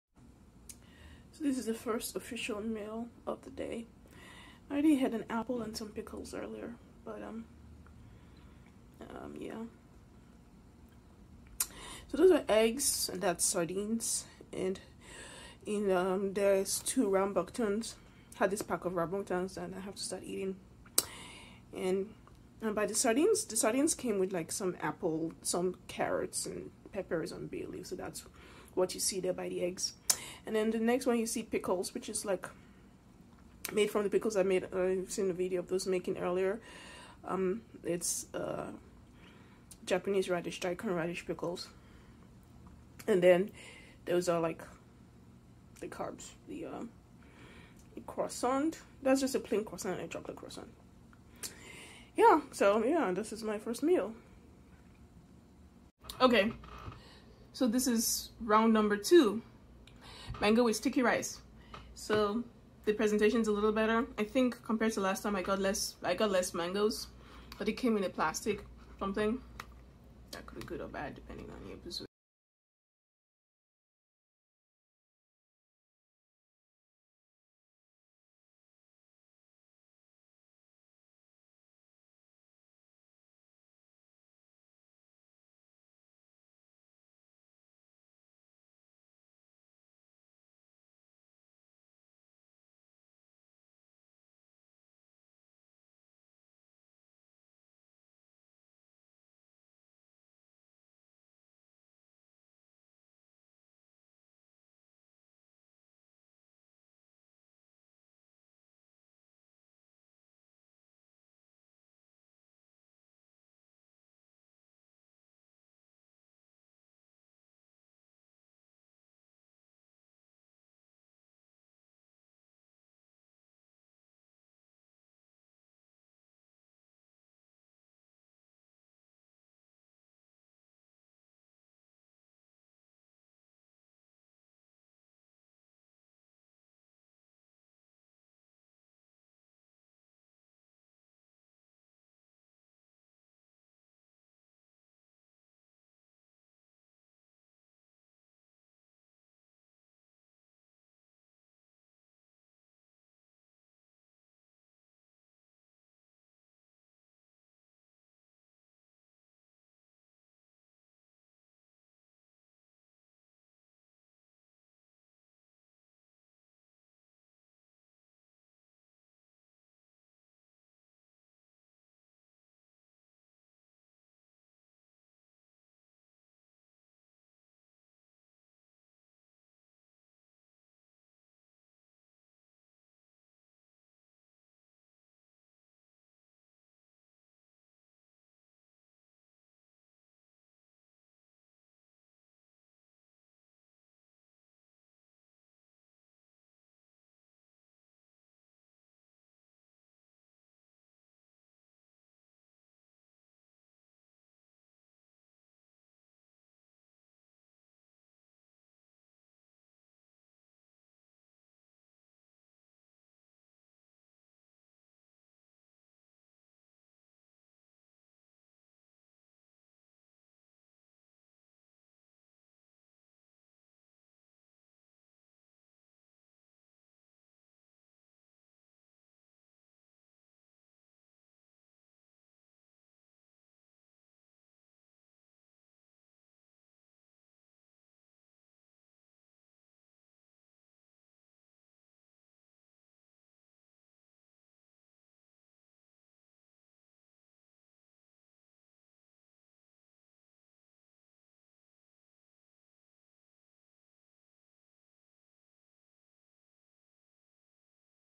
Breaking a 3 day fast. What I eat in a day
Foods I ate after breaking a 3 day fast. Sardines, eggs, pate ...